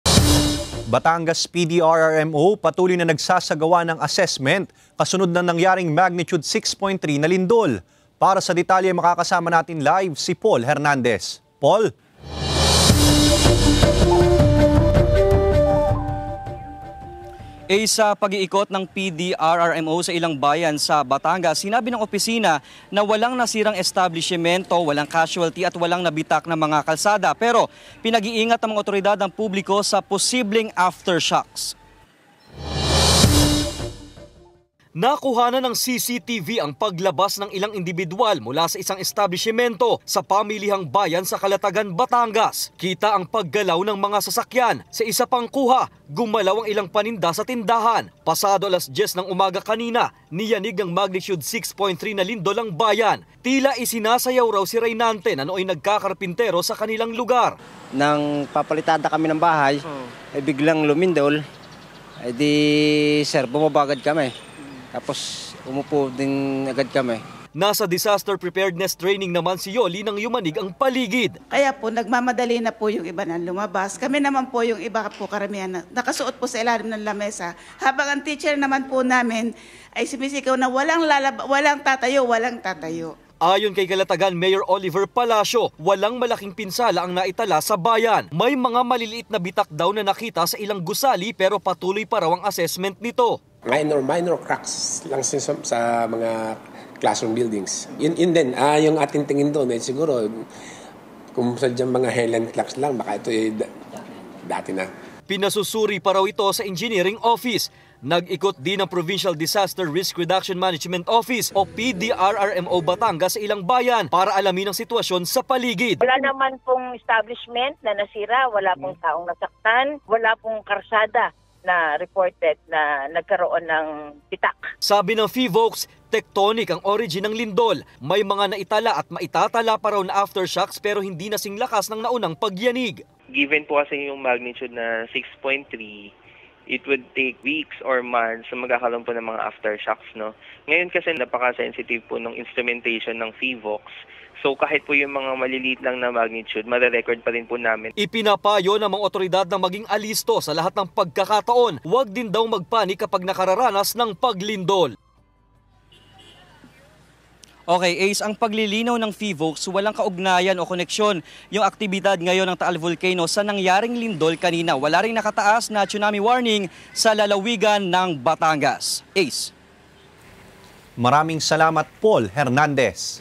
Balitang Southern Tagalog: Earthquake Alert
Calatagan, Batangas, niyanig ng magnitude 6.3 na lindol; Batangas PDRRMO, patuloy na nagsasagawa ng assessment kasunod ng nangyaring lindol.